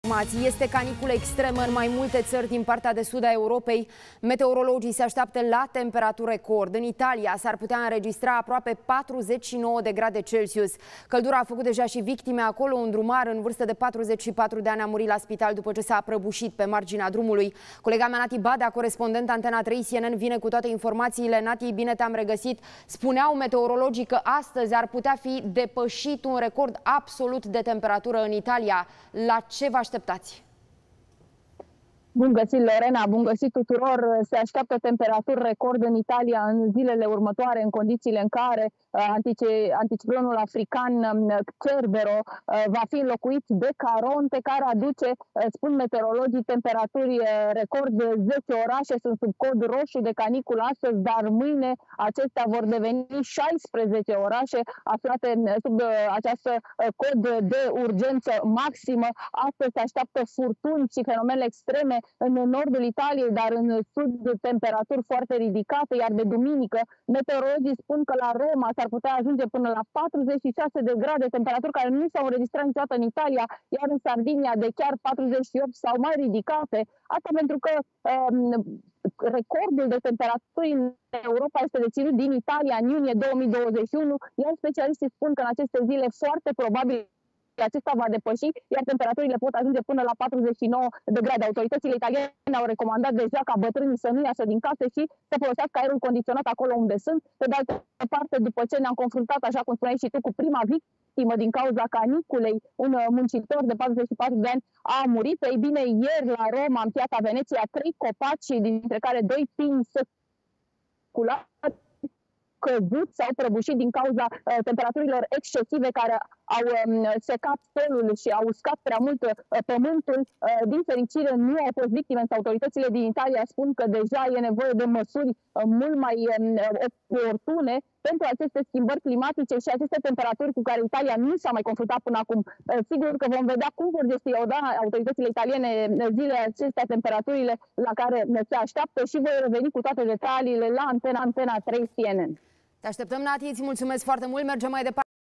Cerberus, fenomenul meteo care topeşte Europa. Meteorologii se așteaptă la temperaturi record
Este caniculă extremă în mai multe țări din partea de sud a Europei. Meteorologii se așteaptă la temperaturi record de 49 de grade celsius.